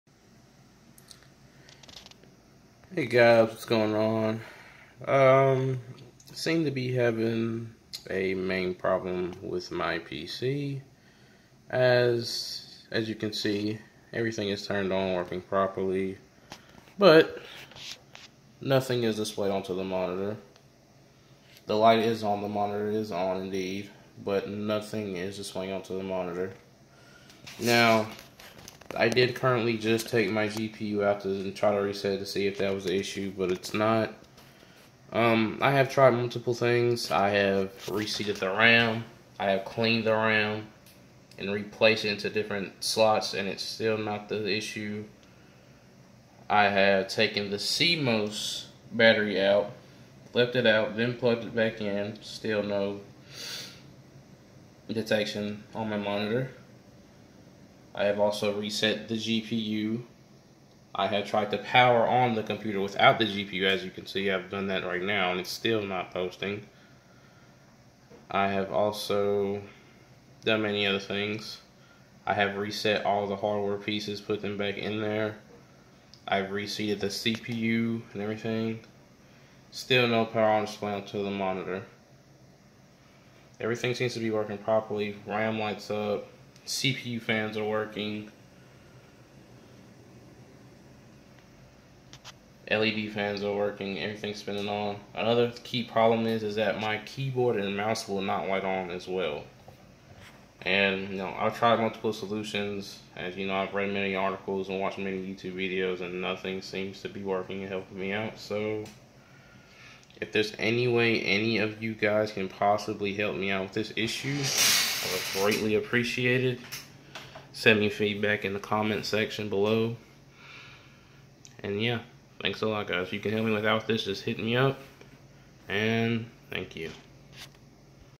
HELP!, PC turning on but no display to monitor or mouse and keyboard (FIXED!!)
Hey guys, asking any helpful viewers for a favor and that is to help consult with my issue on my computer. Thank you guys so much!